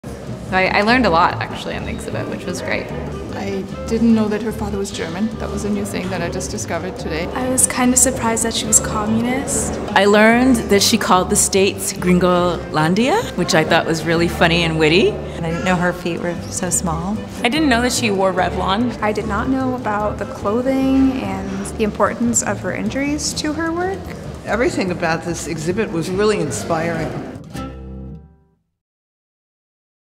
Visitors on Frida Kahlo [Part 2]
Gain a deeper understanding of Frida Kahlo’s cultural heritage, physical disabilities, and political beliefs—all of which were at the heart of her work—in the largest U.S. exhibition in ten years devoted to the iconic painter.

Frida Kahlo: Appearances Can Be Deceiving
February 8–May 12, 2019